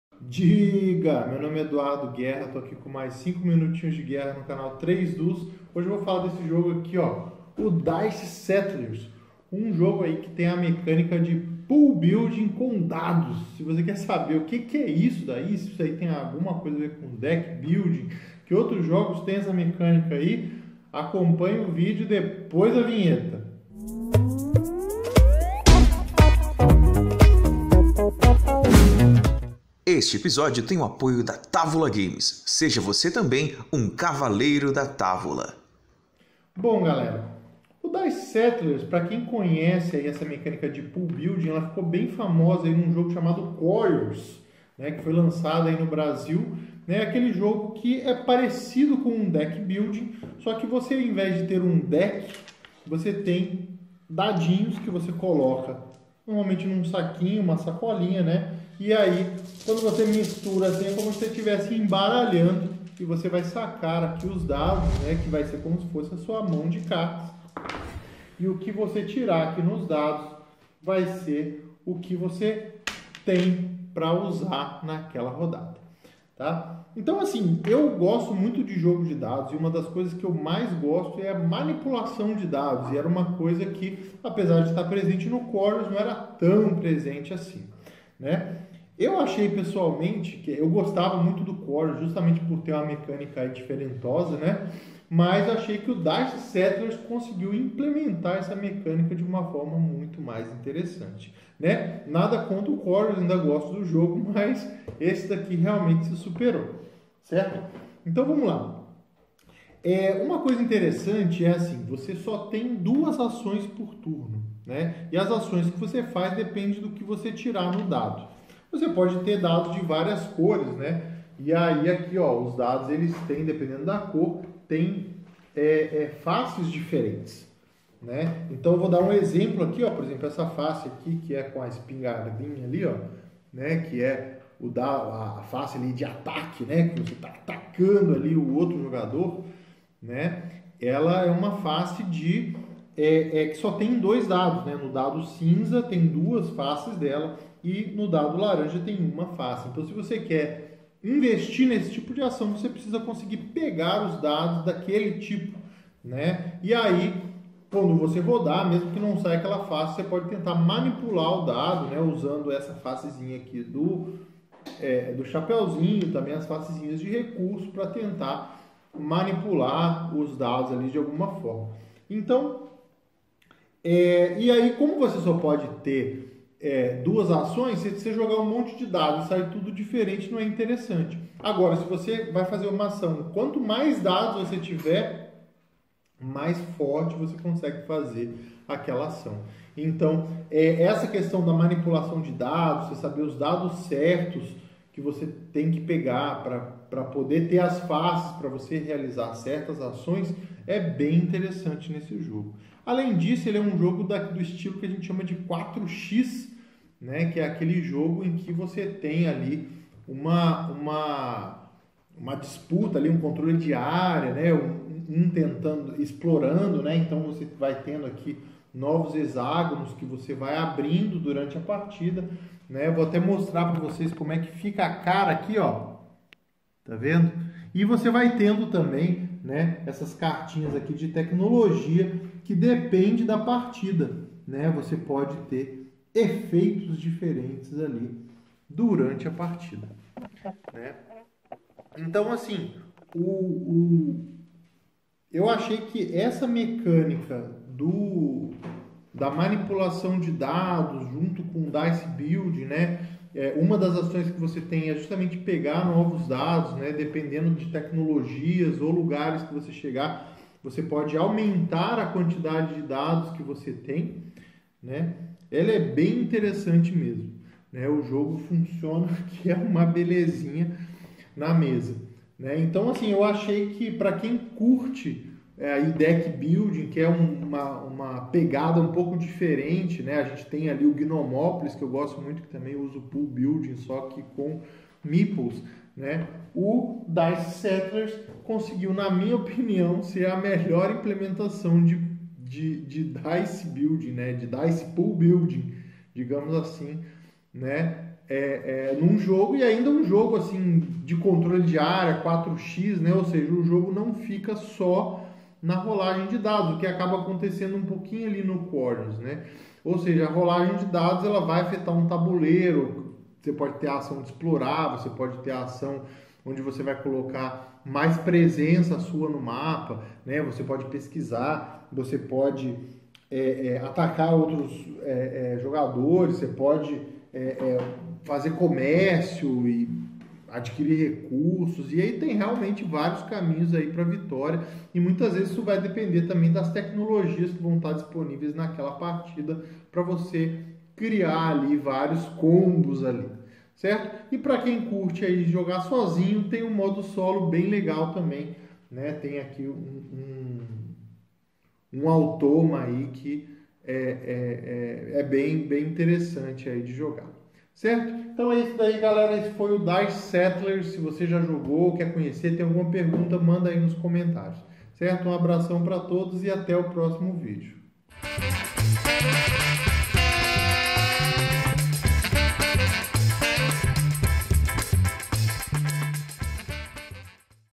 Dice Settlers - Review - 5 Minutos de Guerra - Ep. 272
Edu Guerra apresenta Dice Settlers, neste vídeo ele fala do jogo e da suas considerações. A principal mecânica dele é pool building, onde neste caso é constituído de dados. Sem falar na arte linda desse jogo. Confere aí!